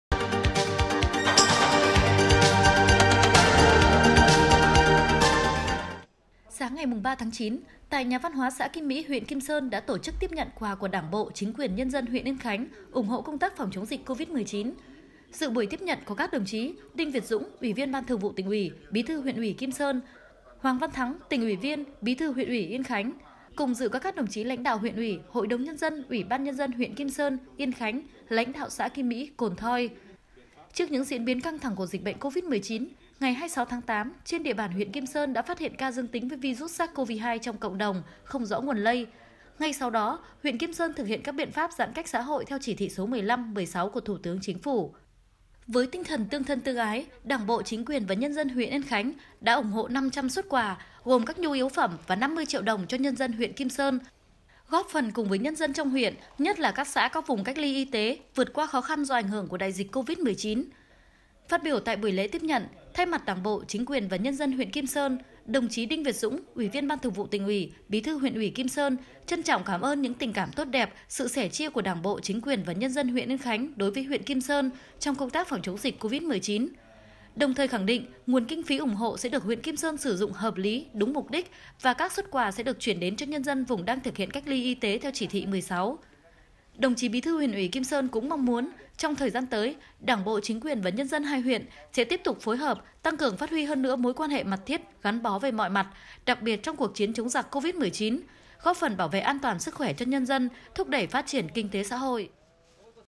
Kim Sơn tiếp nhận ủng hộ phòng, chống dịch COVID-19
Sáng 3/9, tại nhà văn hóa xã Kim Mỹ, huyện Kim Sơn đã tổ chức tiếp nhận quà của Đảng bộ, chính quyền, nhân dân huyện Yên Khánh ủng hộ công tác phòng, chống dịch COVID-19.